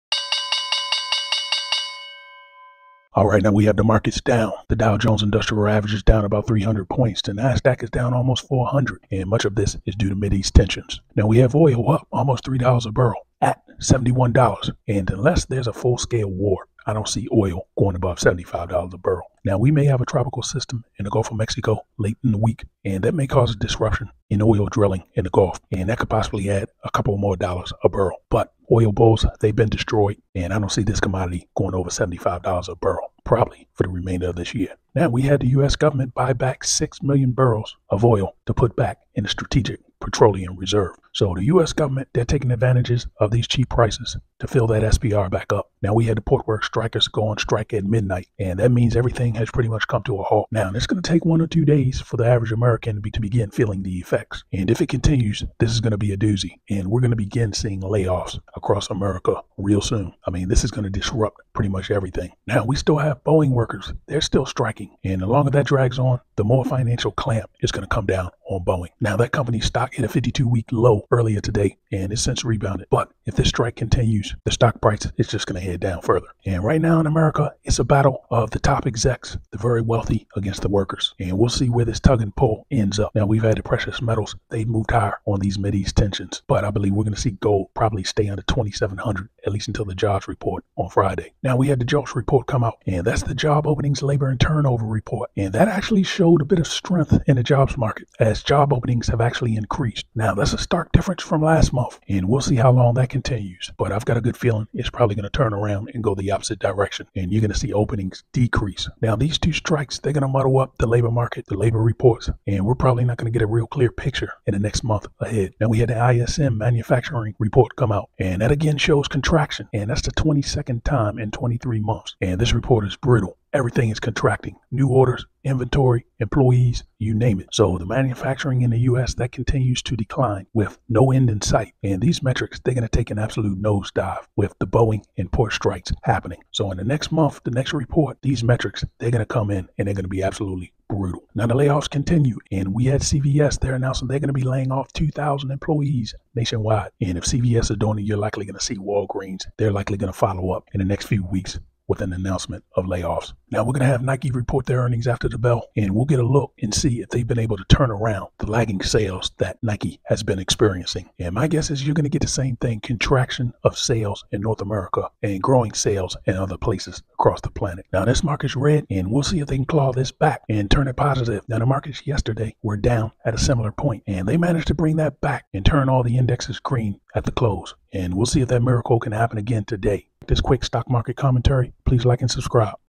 Mid-day Market Report October 1st!
Quick mid-day commentary on financial news and data!